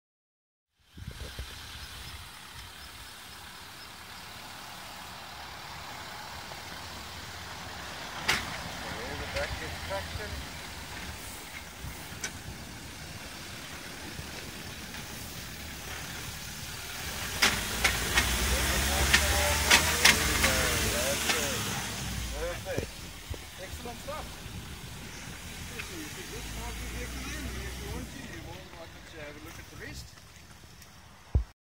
Defender getting cross axled
Land Rover Experience with the Disco 4 at Simonsig Wine Estate, Stellenbosch, 7th Nov 2015